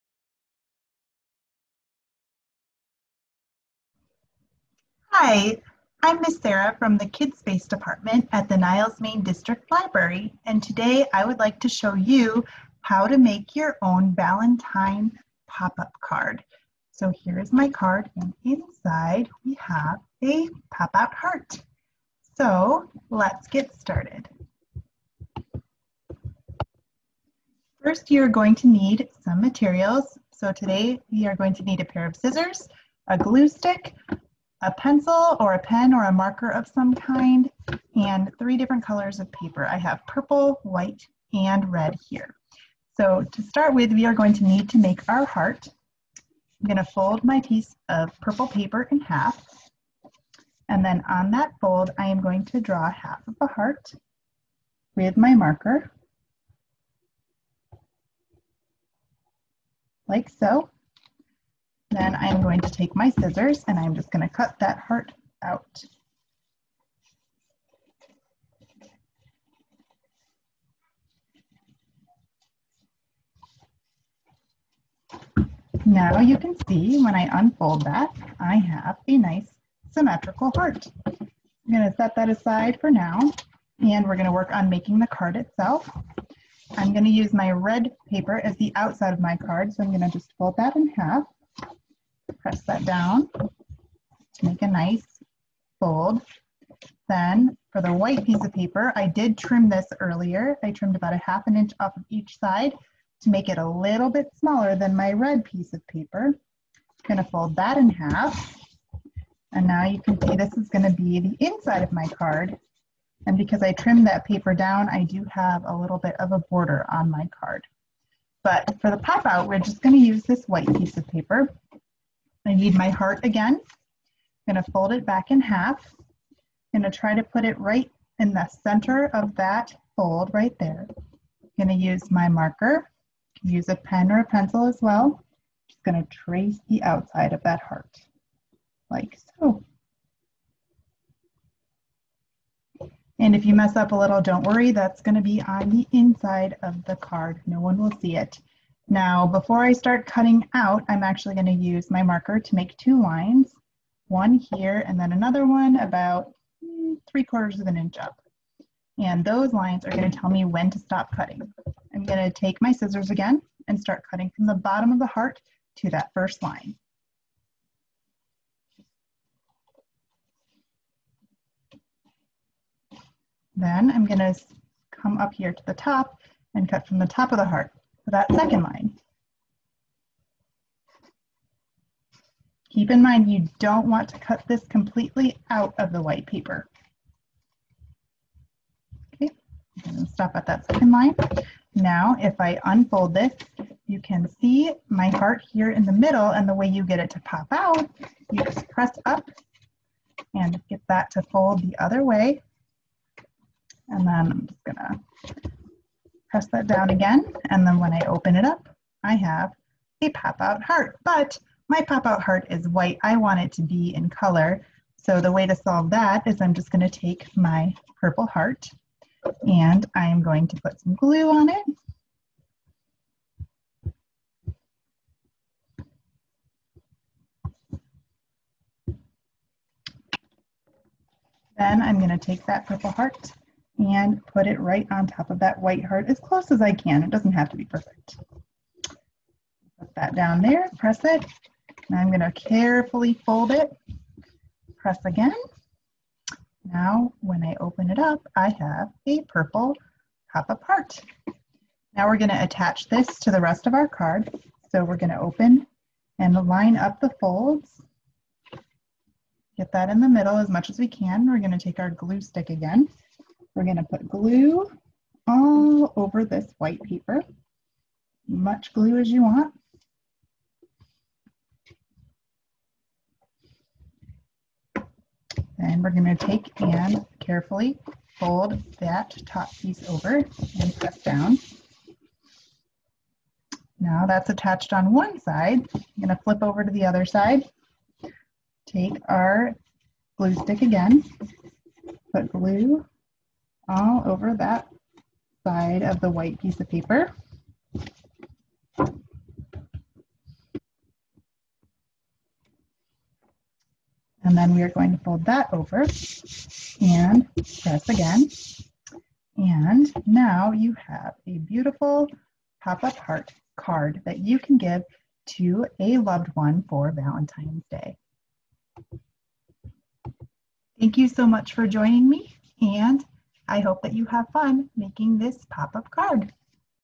Valentine Pop Up Card
Sarah from Youth Services demonstrates how to make a Valentine pop up card.